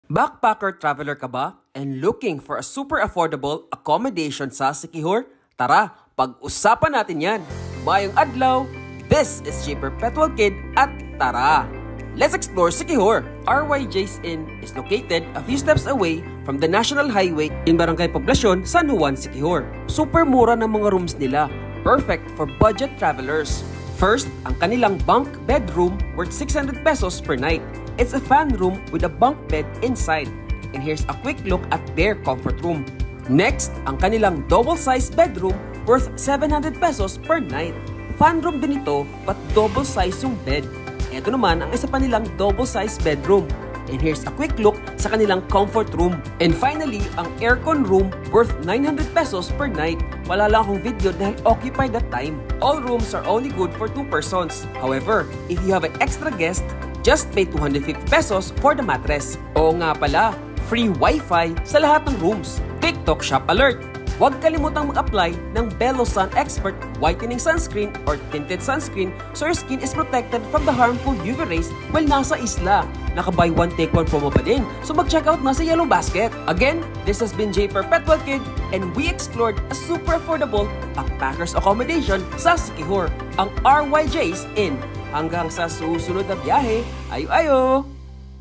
RYJ's Inn | A Backpacker's Haven in Siquijor
RYJ's Inn is a backpackers haven nestled in the heart of Barangay Poblacion, San Juan, Siquijor. Offering a warm and welcoming atmosphere, this charming inn is the perfect choice for budget travelers.

With affordable rates starting at only Php 600-700 per night for a fan room and Php 900 per night for an air-conditioned room, RYJ's Inn provides comfortable accommodations without breaking the bank. Situated just a few minutes away on foot, the stunning white sand beach of San Juan awaits, inviting guests to relax, swim, and soak up the tropical sun.

Whether you're seeking adventure or tranquility, RYJ's Inn is an ideal base for exploring the beauty of Siquijor while enjoying a cozy and affordable stay.